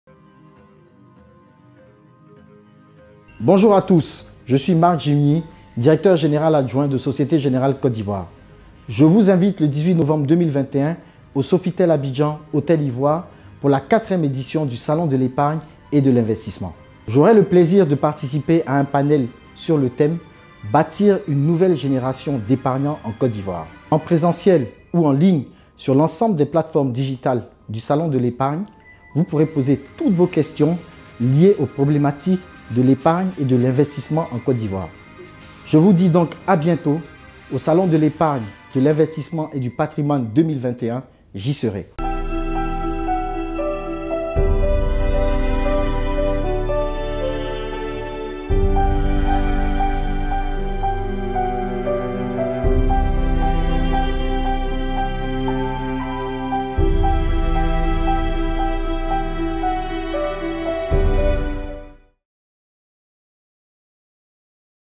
Rendez-vous au SEIP 2021 avec Marc GIUGNI
Rejoignez-nous au SEIP 2021 avec Marc
GIUGNI, Directeur Général Adjoint - Directeur Clientèle Entreprises, Société Générale Côte d’Ivoire. Il interviendra sur la session plénière numéro 1 : « Bâtir une nouvelle génération d’épargnants en Côte d’Ivoire».